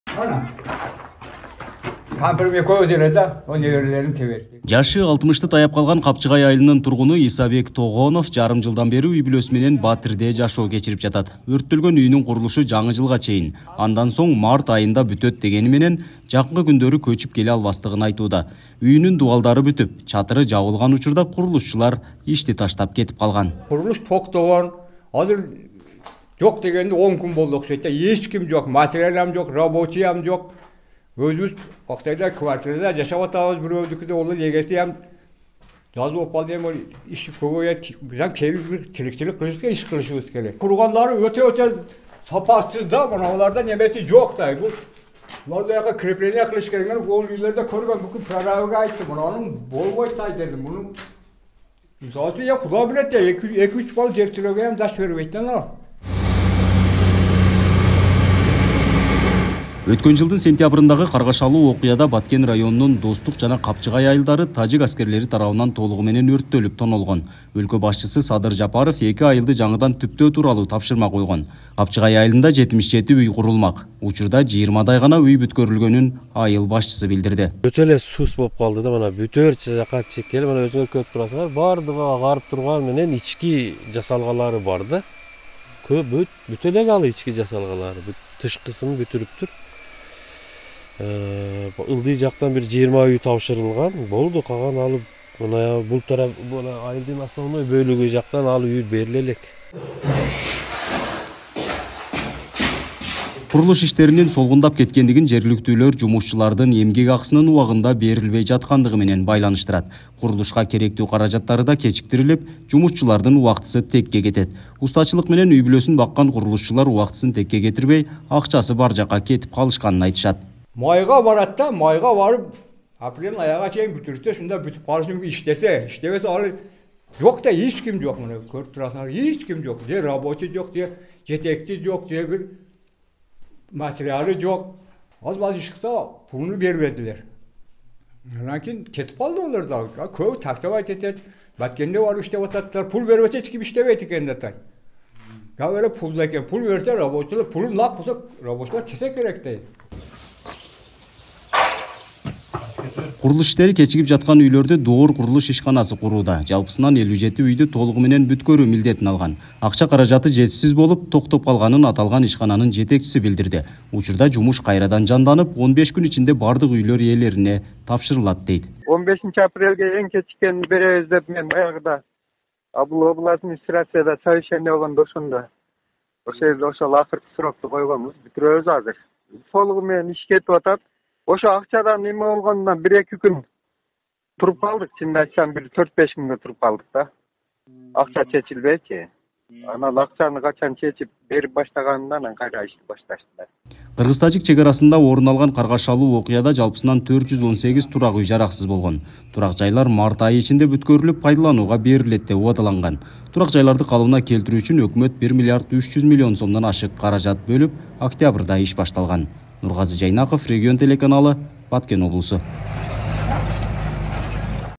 Баткен: Усталар акчасын алалбай кетип калды
Атайын генплан менен жаңыдан түптөлүп жаткан чек арадагы Капчыгай айылында үйлөрдүн курулуш иштери суздап калганын жергиликтүүлөр айтышууда. Жалпысынан 77 үйдүн болгону 20дайы гана бүткөрүлгөн. 57 үйдү куруу милдетин алган “Доор курулуш” ишканасы куруучулардын эмгек акысын бербей жатат деген дооматтар да айтылууда. Бийлик тарап март айынын аягына чейин өрттөлгөн үйлөрдүн курулушу  толугу менен аяктайт деп билдирген.